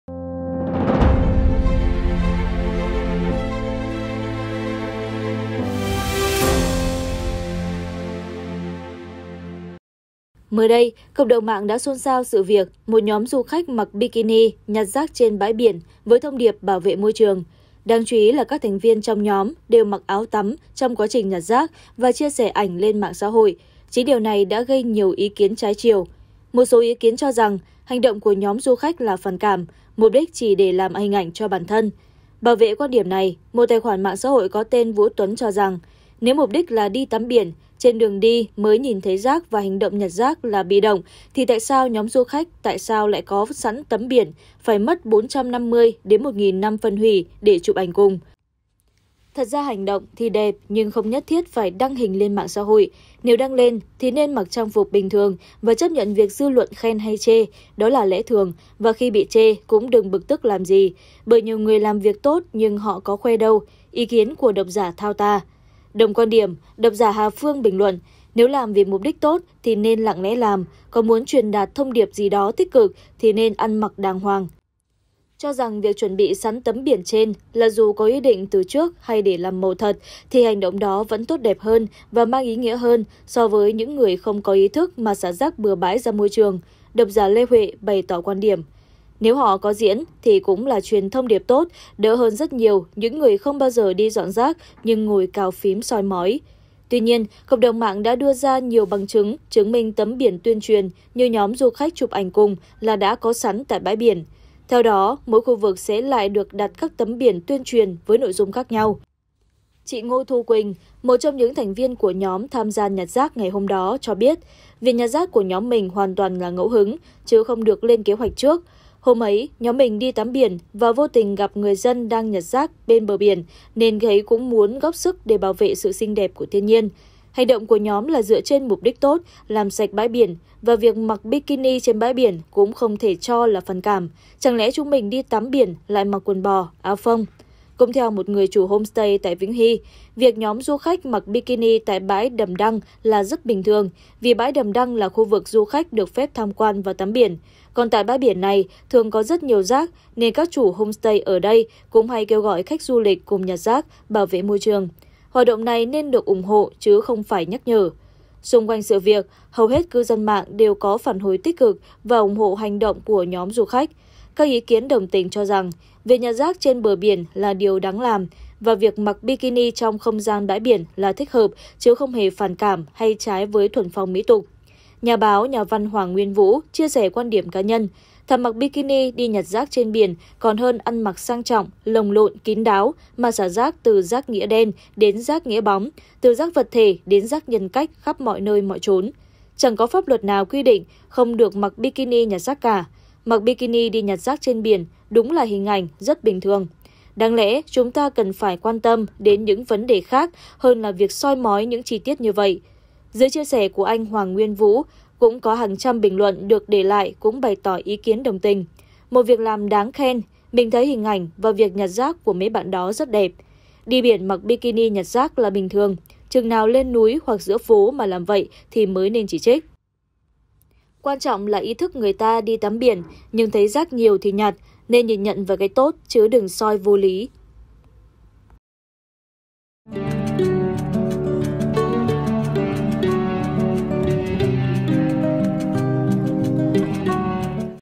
Phản Cảm Mặc Bikini Nhặt Rác Bị Chê: Mặc Đẹp Xả Rác Bừa Bãi Mới Đáng Chê! | Báo Dân Việt
Báo Dân Việt | Sau sự việc nhóm du khách mặc bikini nhặt rác tại bãi biển, cộng đồng mạng đã chia thành hai luồng ý kiến. Một số người bày tỏ, nếu thực sự bị nhắc nhở thì sẽ không ai tình nguyện làm việc này nữa.

Mời quý vị và các bạn xem thêm: Nước Mắt Người Thân Từ Biệt 3 Chiến Sĩ Cảnh Sát Chữa Cháy Hy Sinh Khi Làm Nhiệm Vụ